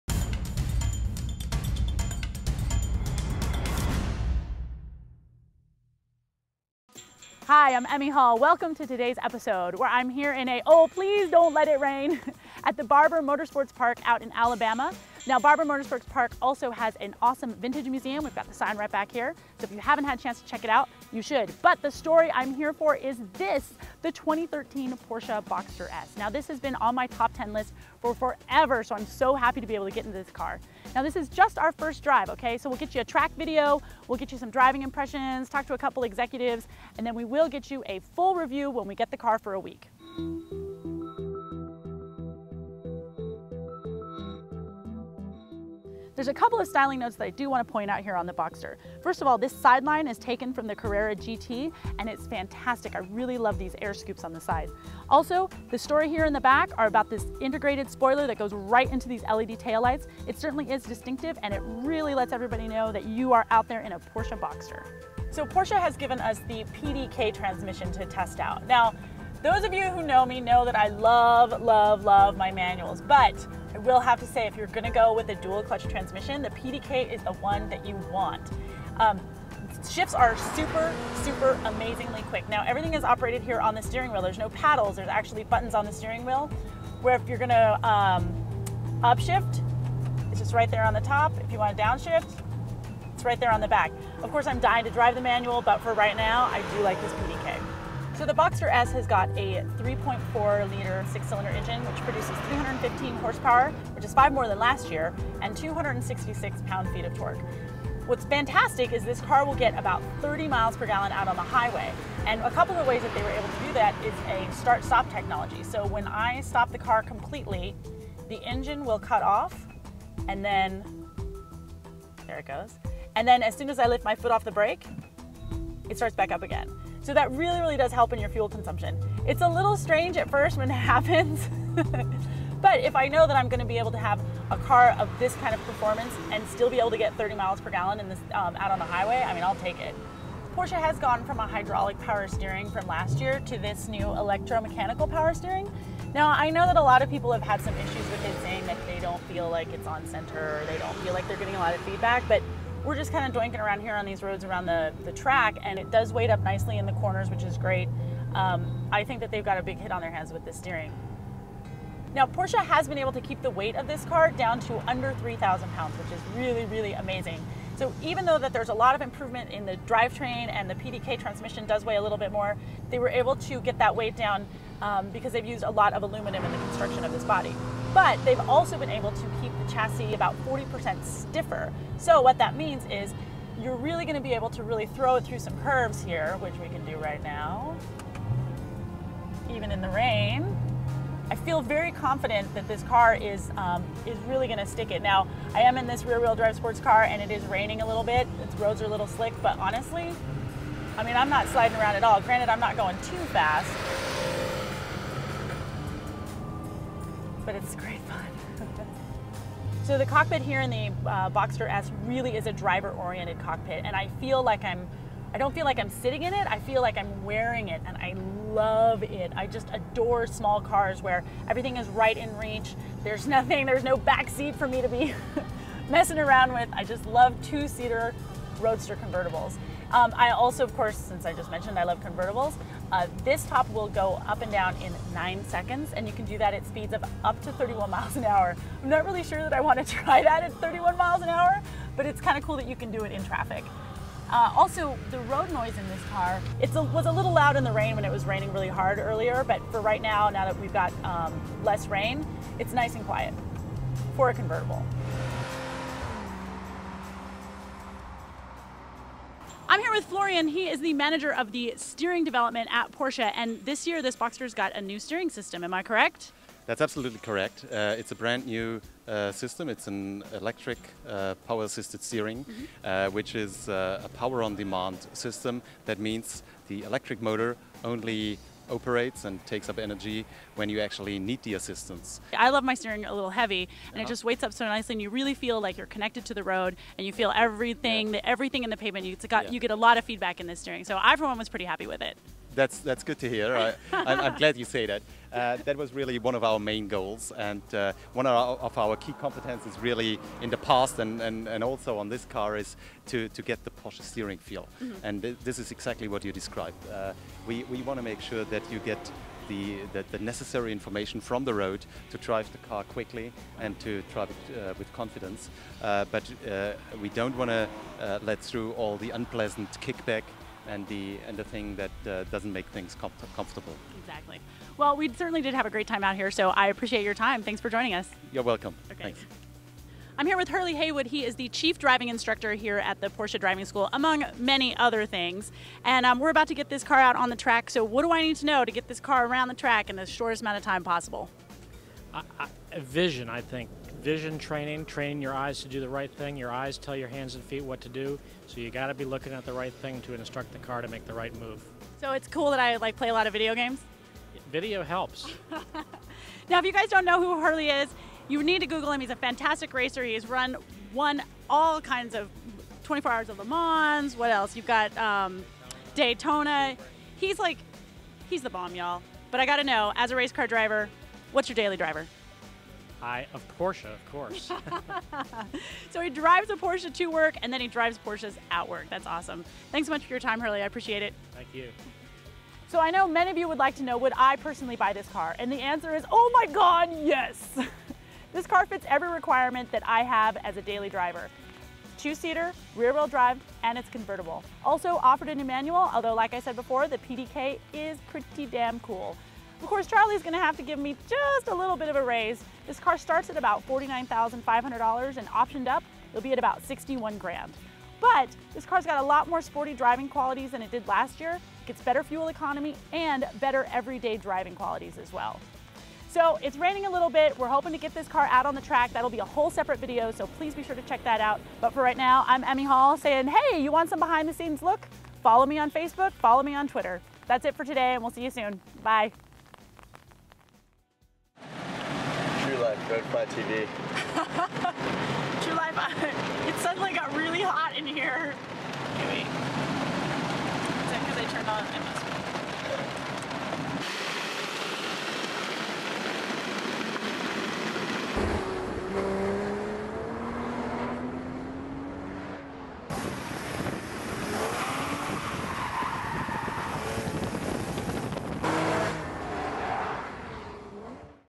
Porsche Boxster S 2013 First Test Drive & Car Review with Emme Hall by RoadflyTV
The new 2013 Porsche Boxster rides on a chassis derived from the current generation 991 Porsche 911. The new Boxster has also been on a diet and is about 55 lbs lighter than the previous model.

To see additional pictures and the window sticker for the 2013 Porsche Boxster S

Interviews include: Porsche engineer Florian Sprenger and Hurley Haywood

Host: Emme Hall

Vehicle: 2013 Porsche Boxster S
Trim package: S, Convertible/Cabriolet
Base Price Standard Porsche Boxster: $49,500
Base Price Porsche Boxster S: $61,850
Engine: Boxer 6-cylinder, Mid-engine, 3.4L
Transmission: PDK, 7-speed
Chassis code: 981
Horsepower: 315 hp @ 6700 rpm
Torque: 266 lb-ft @ 4500 rpm
0-60 MPH: 4.8 seconds
1/4-mile time:
Top speed: 170+ MPH
Curb weight: 2960 lbs.
Warranty: 4-years/50000-mile warranty
Fuel economy (EPA miles per gallon rating):  city 20 / 30 highway MPG (Estimated)